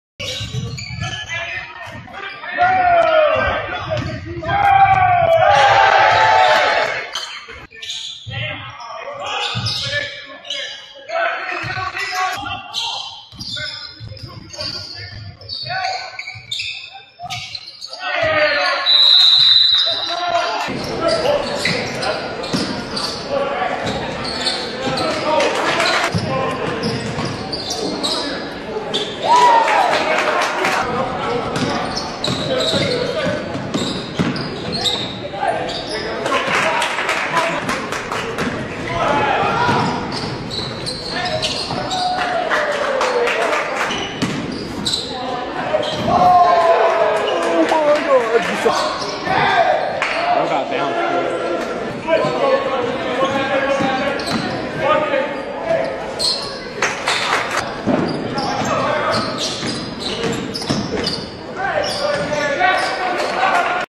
TJ ASHFORD | Black Jersey #0 | PG | Southwestern College J vs Bethel & Bethany College (KS) | 23 -24
Mid Season College Highlights from two games ( Red Shirt Freshman) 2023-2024 season

Bethel College video was frozen after 1st half.. 17 pnts

Bethany College 19pnts